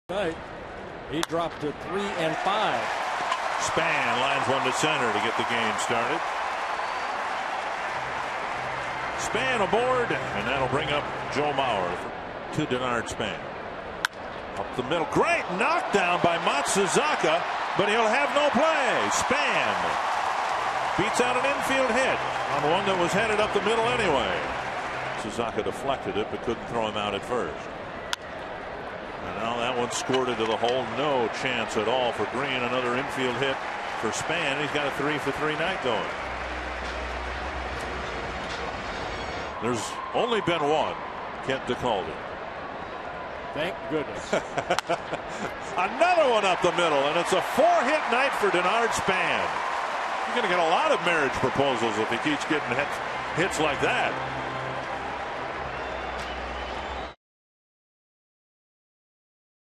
2009/05/27 Span's four hits
BOS@MIN: Span ties a career high with four hits